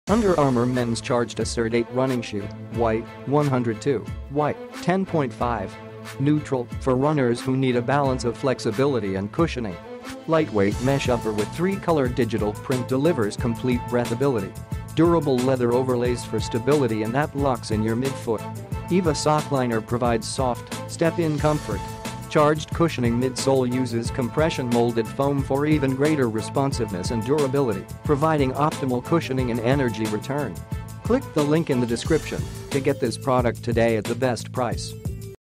Under Armour Men's Charged Assert 8 Running Shoe, White (102)/White, 10.5
NEUTRAL: For runners who need a balance of flexibility & cushioning
Lightweight mesh upper with 3-color digital print delivers complete breathability
Durable leather overlays for stability & that locks in your midfoot
EVA sockliner provides soft, step-in comfort
Charged Cushioning midsole uses compression molded foam for even greater responsiveness & durability, providing optimal cushioning & energy return